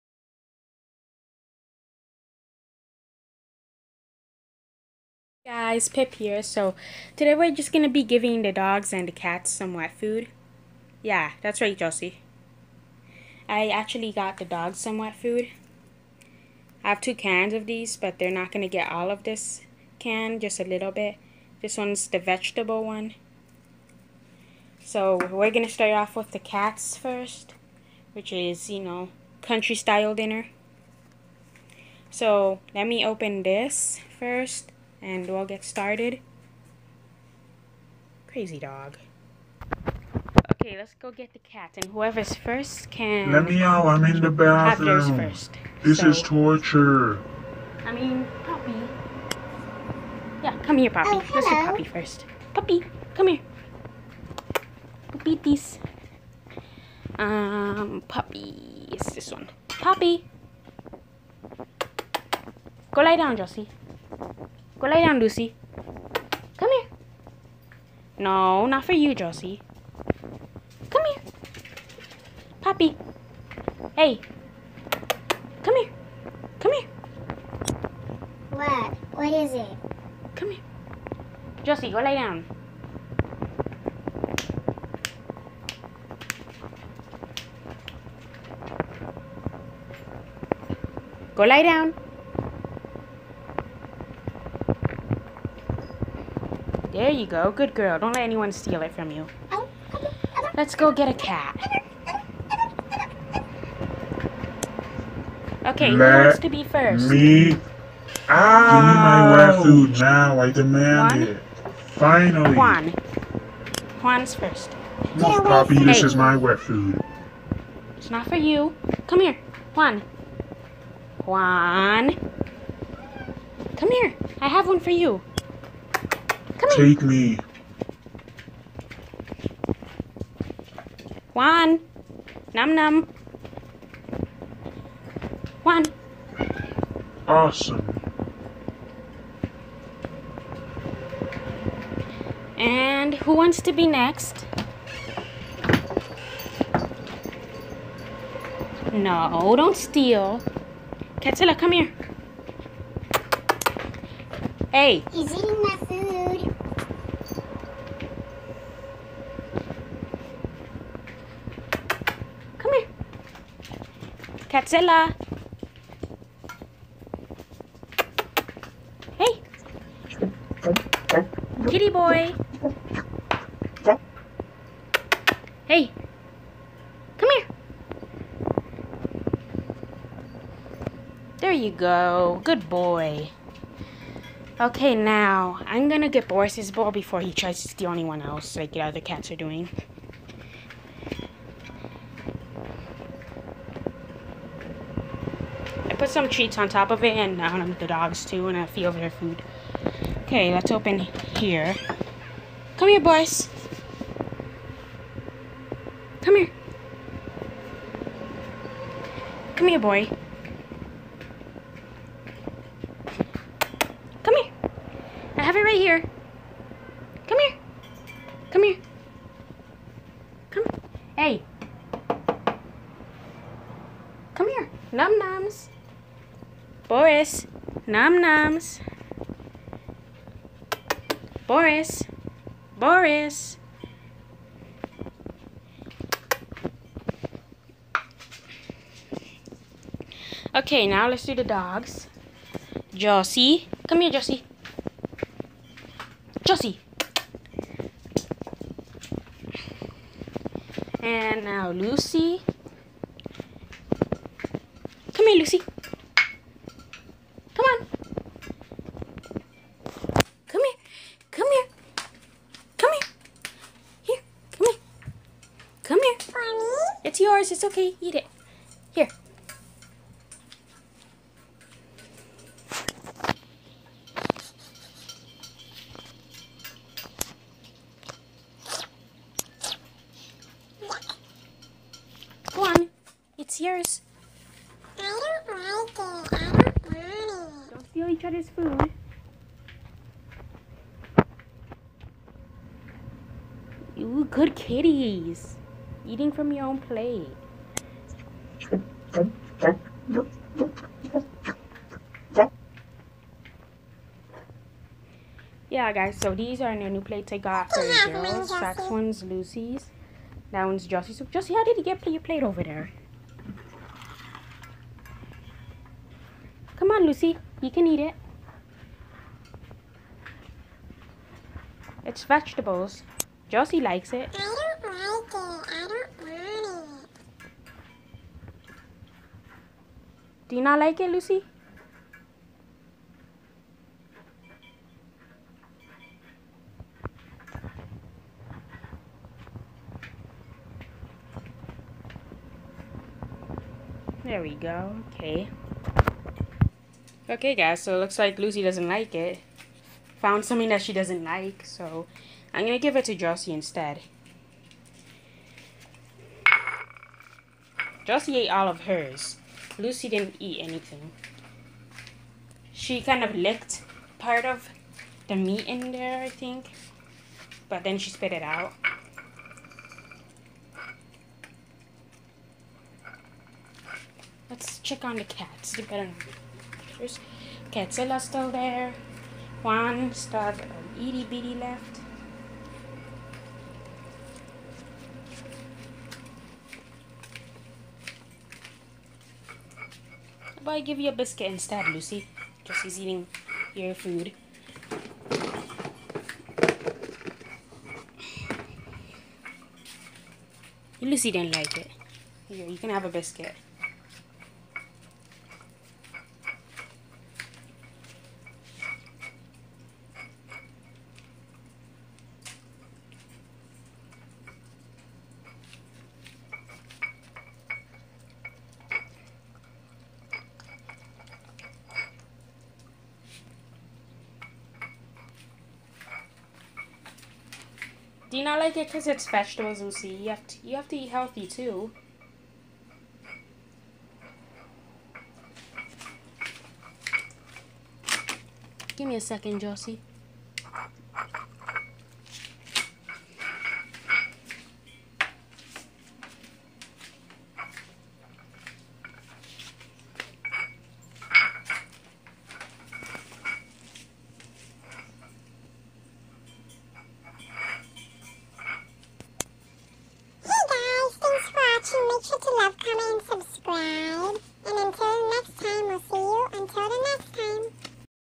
Lucy The Talking Dog S2 Ep12- Wet Food 2
Hi Guys In This Video Me,Josie,Boris,Juan,Catzilla,Poppy Get Wet Food and I Dont Like It Yuck!!!